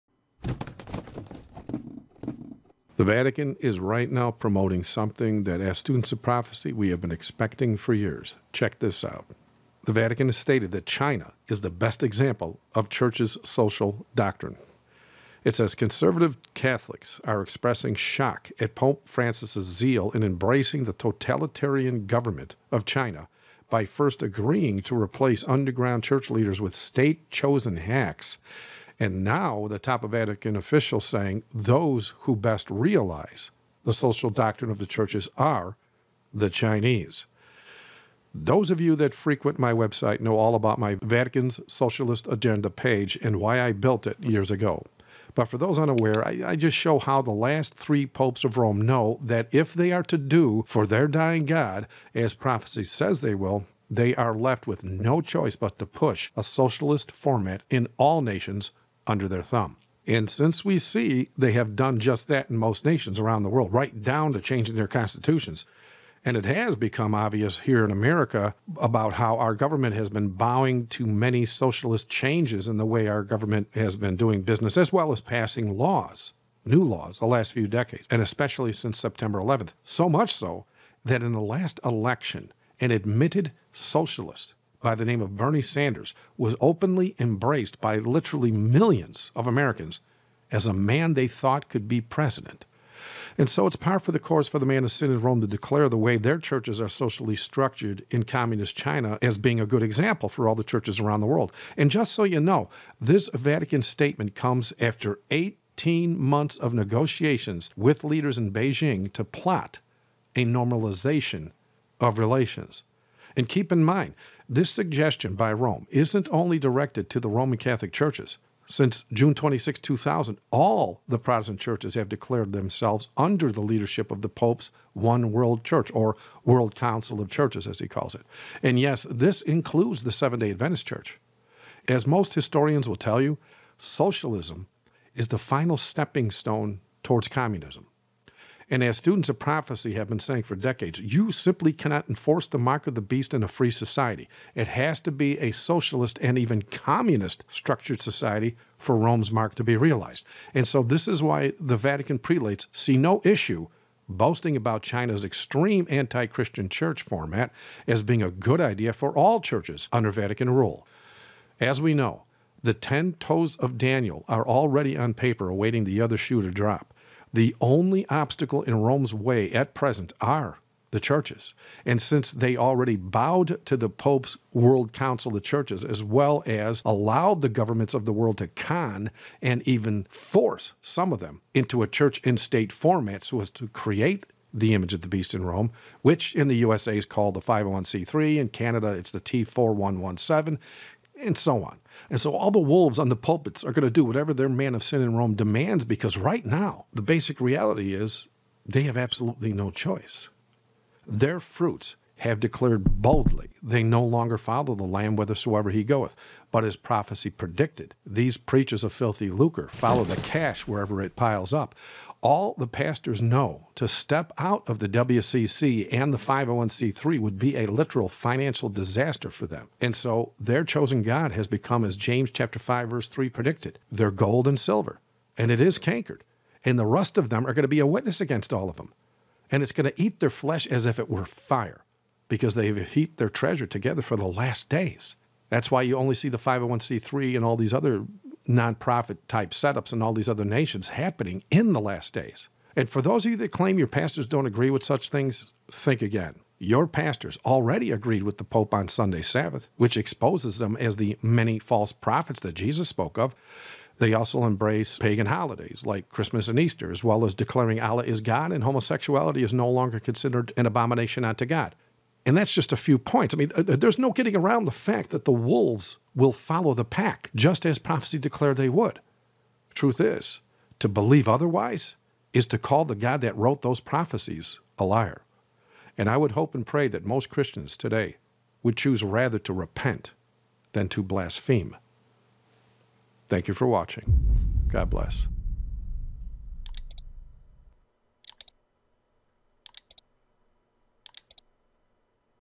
Vatican Promotes a Communist Church Structure!
As expected as it is for enforcing the mark, for some it's still a shock for the Pope to admit

(like the last three have in writing) that they want a Socialist format in both governments and

Churches. As prophesied, things are in fact SPEEDING UP!

All links to the videos and articles mentioned in video are in today's poGm blog entry